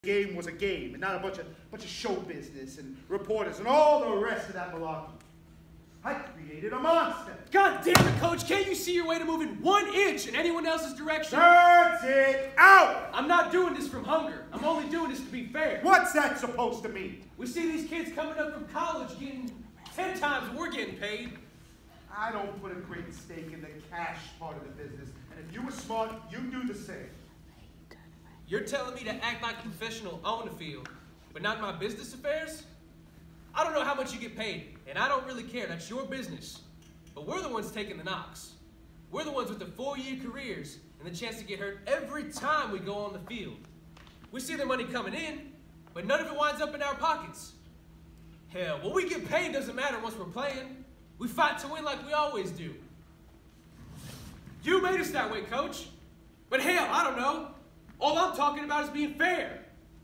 Lombardi: Contract Negotiation Scene
‘Lombardi’ is a play by Eric Simonson. Following the story of The Green Bay Packers and some of their star players. This play gives the audience an inside look into Vince Lombardis life as he manages being a husband and a NFL coach. The players featured in this story are Jim Taylor, Paul Hornung, and Dave Robinson. The story gives us a point of view through the reporters side of the story as well featuring Michael McCormick who interviews Coach Lombardi and the other players. This production of the play took place in Bethlehem, PA.

Actors:
Vince Lombardi - Bob Wright
Jim Taylor - George Roman
Dave Robinson - Justin Ferguson
Paul Hornung - Blake Kreider
Michael McCormick - Ryan Allen